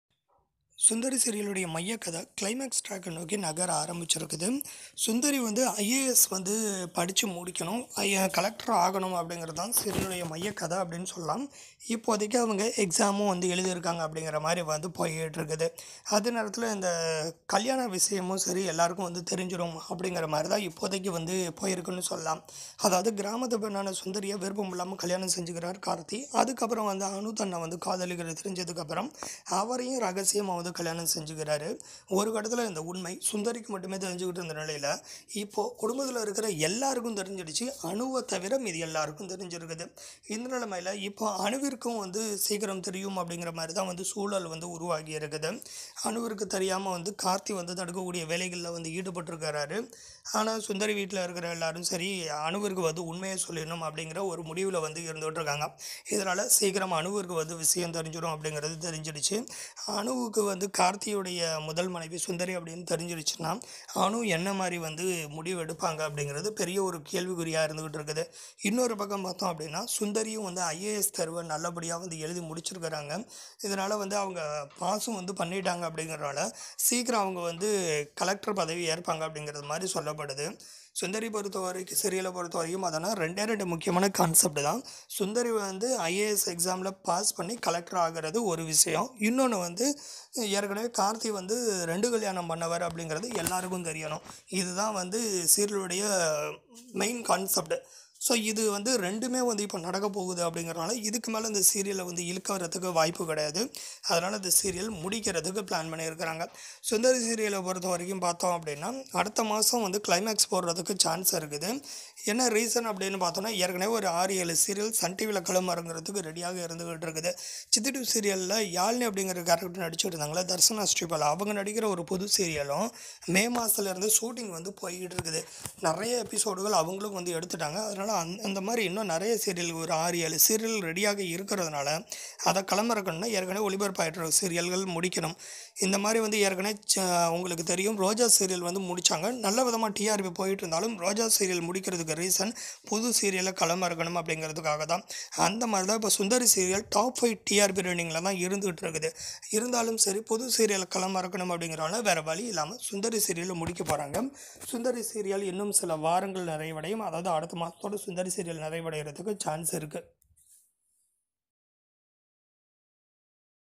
இறுதிகட்டத்தில் சுந்தரி | Anu joins Sundari when he learns the truth about Karthik | Sundari Review
இறுதிகட்டத்தில் சுந்தரி | In this video I talk about the Anu joins Sundari when he learns the truth about Karthik | Sundari Review